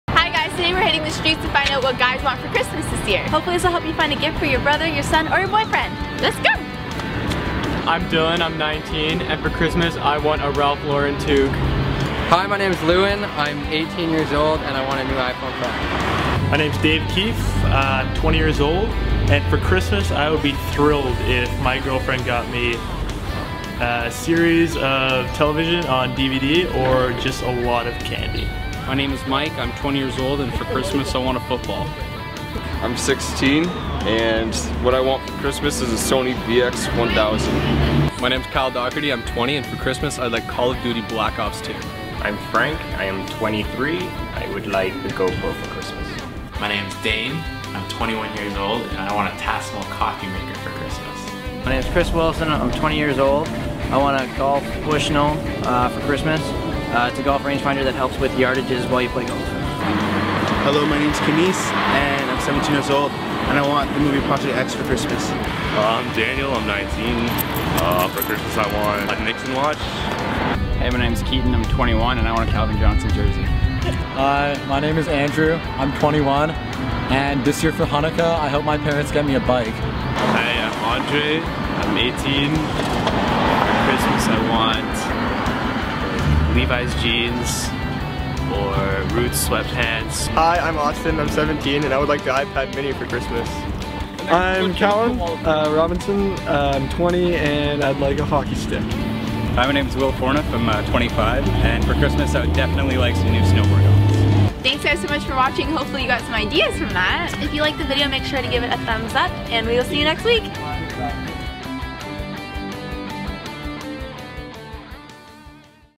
WHAT GUYS WANT FOR CHRISTMAS: BY GUYS!
We hit the streets of Toronto and asked some guys what they want for Christmas- or Hanukkah - this year!
Hopefully this will give you an idea of what to get your boyfriend (or son, or brother)!
Thanks to all the guys that appear in our video and let us film them :)